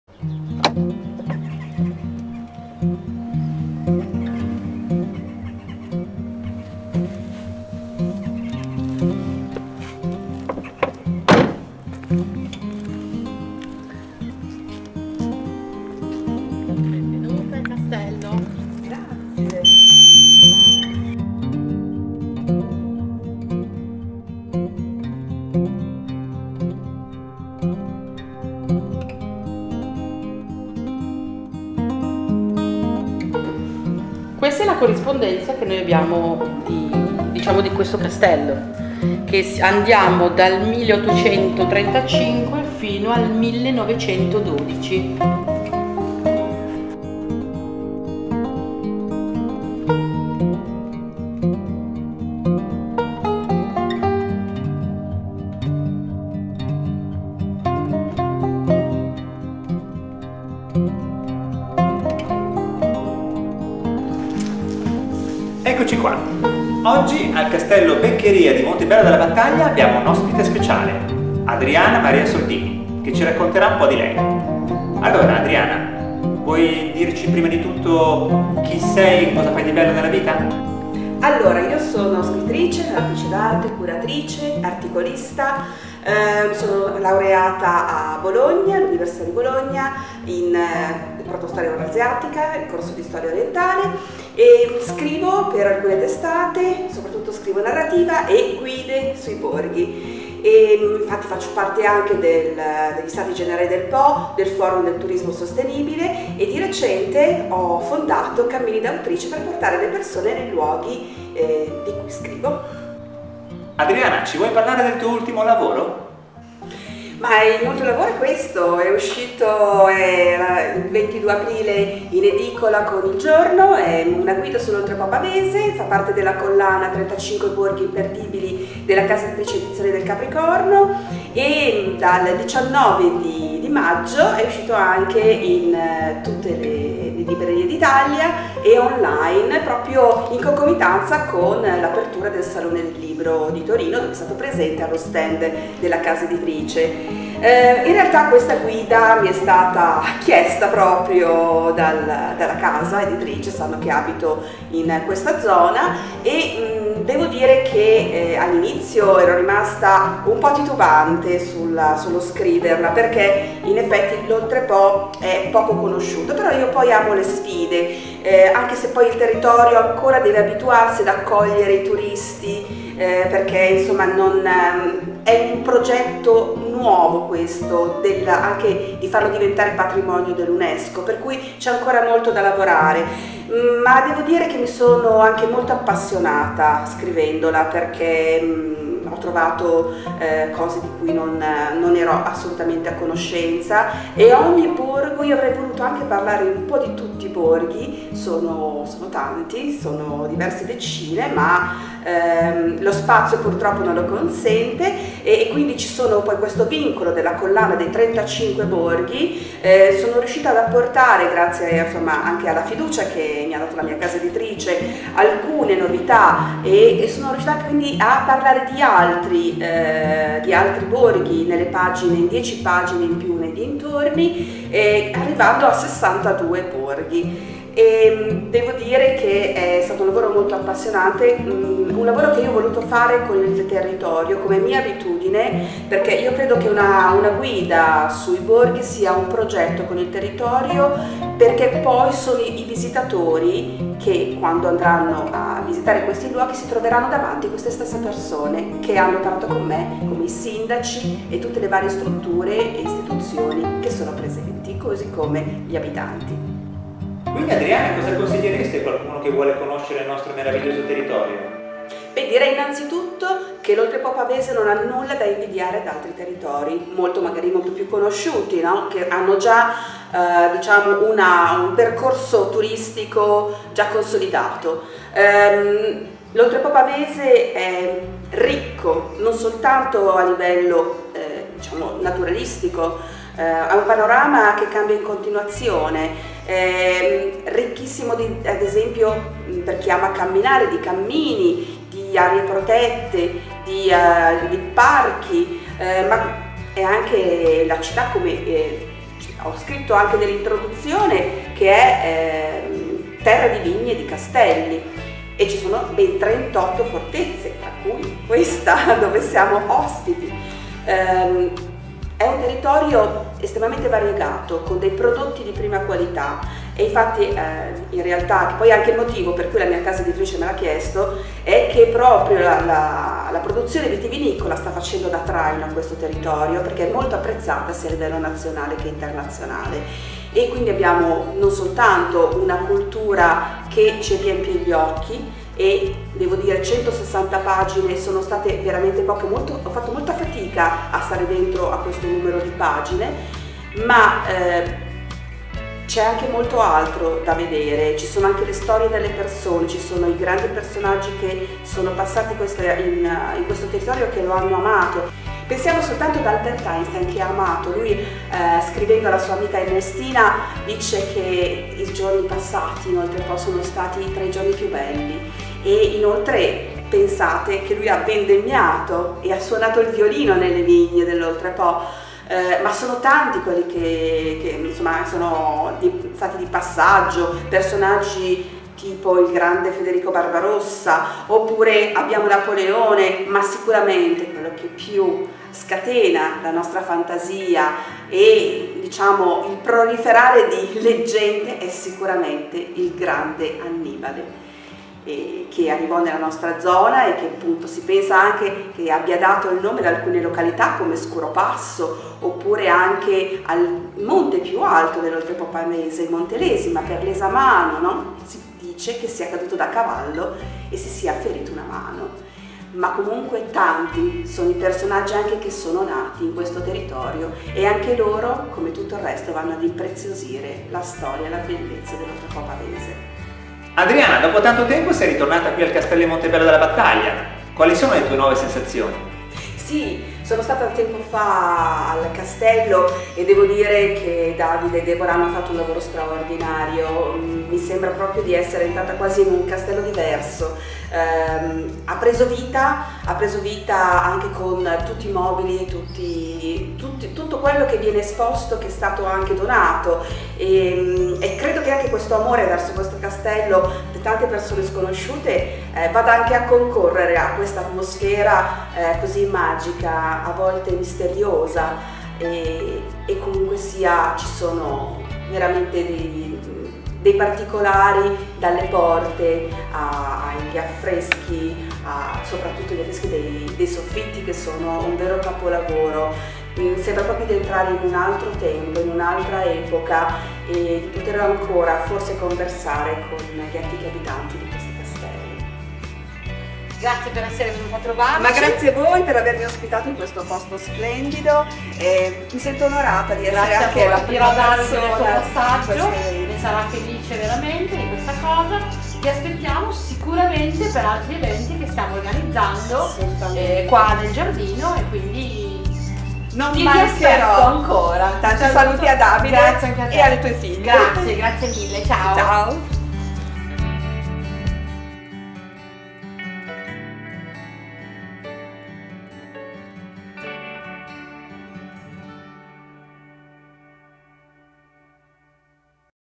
Adriana Maria Soldini in visita al Castello Beccaria di Montebello della Battaglia.
C'è una terra sorprendente circa 60 km a sud di Milano, a cui è facilissimo accedere: basta attraversare il ponte sul Po. Quello è il varco da cui si entra in una storia avvincente, ancora in gran parte da scoprire: l'Oltrepò Pavese. Inzia cosi la prefazione dell'ultimo lavoro di Adriana Soldini "35 Borghi Imperdibili l'Oltrepò Pavese" che oggi visita il Castello Beccaria di Montebello della Battaglia.
Laureata in protostoria euroasiatica all'Università di Bologna, Adriana si occupa di molteplici attività  : è scrittrice, articolista, narratrice d'arte e curatrice. Scrive per alcune testate locali: Oltre, bimestrale di cultura, ambiente e turismo e per il Giornale di Voghera dove segue la sua rubrica "Appuntidi una lettrice cronica". Dal 2018 è membro del comitato scientifico degli Stati Generali del Po e dal 2020 fa parte del Forum del Turismo Sostenibile. A novembre 2021 ha fondato Cammini d''Autrice, con il progetto di portare i suoi lettori e fans, nelle terre da lei raccontate per conoscere da vicino storie e costumi delle regioni italiane.
Oggi ci rende visita per ammirare i grandi progressi che si sono compiuti ad oggi nella ristrutturazione del Castello, nel riprisitino di tutte le sue parti e nei lavori per la prossima apertura dell'Enoteca e del Bistrot. La ringraziamo di cuore per aver inserito la nostra realtà nella sua guida sostenedo il progetto.

Davide e Deborah Parisi
Intervista a cura delle Selezioni dell'Oltrepò Pavese